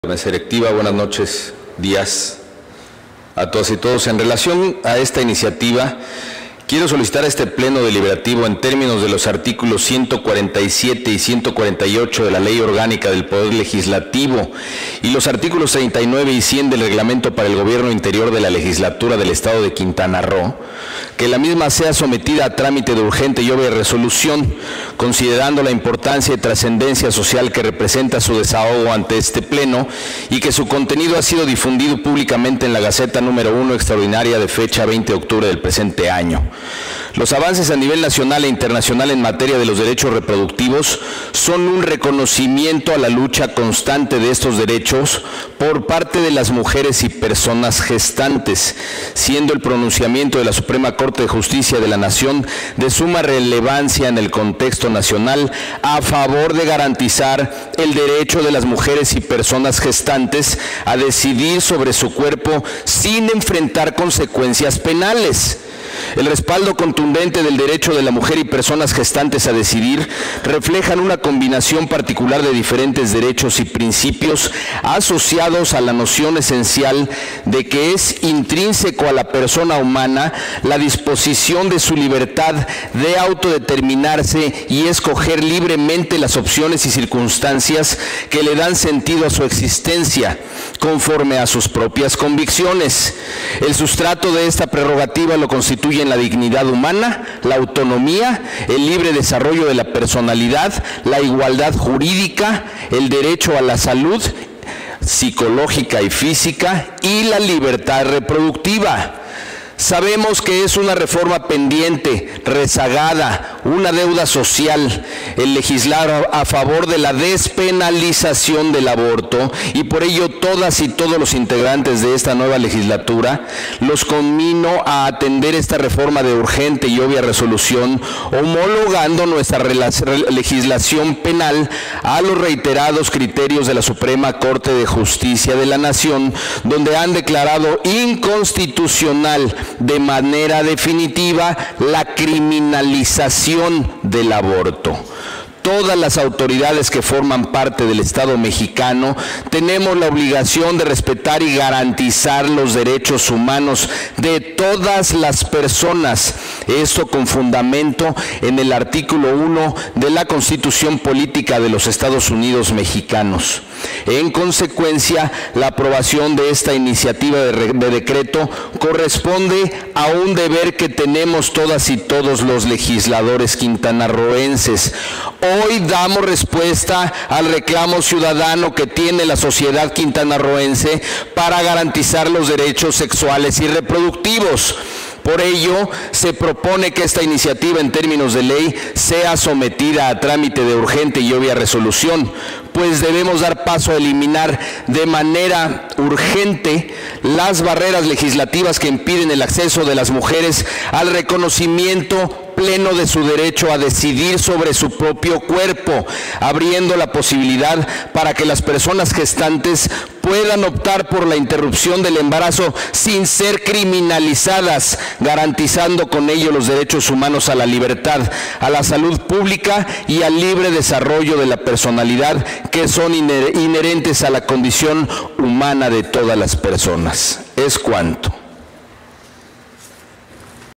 Intervención Dip. Luis Humberto Aldana Navarro. MORENA (26/Octubre/2022)
Intervención del Dip. Humberto Aldana, de MORENA, para solicitar que la iniciativa de decreto por el que se reforman los artículos 92, 93, 94, 95 y 97, en sus fracciones II, III y IV; se adiciona un párrafo segundo y un tercero al artículo 92; y se deroga el artículo 96, todos del Código Penal para el Estado de Libre y Soberano de Quintana Roo; presentada por Diputado Luis Humberto Aldana Navarro, Diputado Ricardo Velazco Rodríguez, Diputada Silvia Dzul Sánchez, Diputada Mildred Concepción Ávila Vera, Diputada Andrea del Rosario González Loría, Diputada María Fernanda Cruz Sánchez, Diputado José María Chacón Chablé, Diputada Elda María Xix Euán, Diputado Omar Antonio Rodríguez Martínez, Diputada Luz María Beristain Navarrete, integrantes del Partido Movimiento de Regeneración Nacional de la H. XVII Legislatura del Estado de Quintana Roo. Publicada en la Gaceta Parlamentaria Año I número 01 extraordinario, de fecha 20 de octubre de 2022; sea tratada de obvia y urgente resolución.